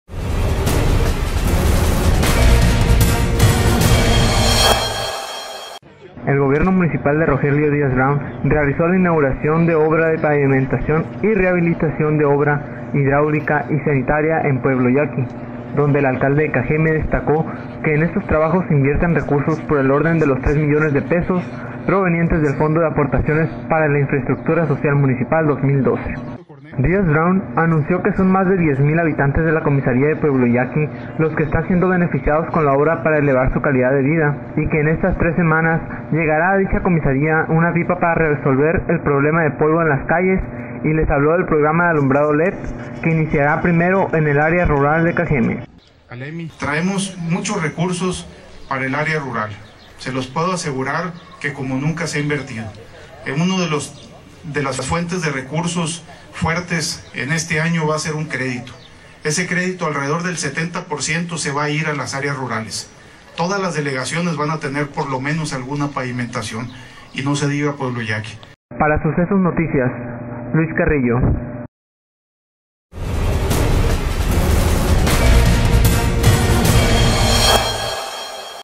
Más de 10 mil habitantes beneficiados con obras de infraestructura en Pueblo Yaqui
Por: Luis Carrillo / Sucesos
el Gobierno Municipal de Rogelio Díaz Brown realizó la inauguración de obra de pavimentación y rehabilitación de infraestructura hidráulica y sanitaria en pueblo yaqui, donde El Alcalde de Cajéme destacó que en estos trabajos se invierten recursos por el orden de los tres millones de pesos, provenientes del Fondo de Aportaciones para la Infraestructura Social Municipal 2012 (FAISM 2012).

Díaz Brown anunció que son Más de 10 mil habitantes de la Comisaría de Pueblo Yaqui los que están siendo beneficiados con la obra para elevar su calidad de vida y que en tres semanas llegará a dicha Comisaría una pipa para resolver el problema de polvo en las calles y les habló del programa de alumbrado LED que iniciará primero en el área rural de Cajéme.

La Directora Técnica de Desarrollo Urbano, Guadalupe Vidales Valenzuela, detalló que la obra consiste en la pavimentación de 4 mil 800 metros cuadrados con concreto asfáltico de las calles Francisco I. Madero, 31 de octubre y gracias a importantes ahorros la calle principal Luis Cajéme.

Entre las actividades realizadas se encuentran la introducción de 280 metros lineales de tubería de PVC, la instalación de 42 tomas y 32 descargas domiciliarias, así como la construcción de 892 metros lineales de guarnición de concreto tipo "L".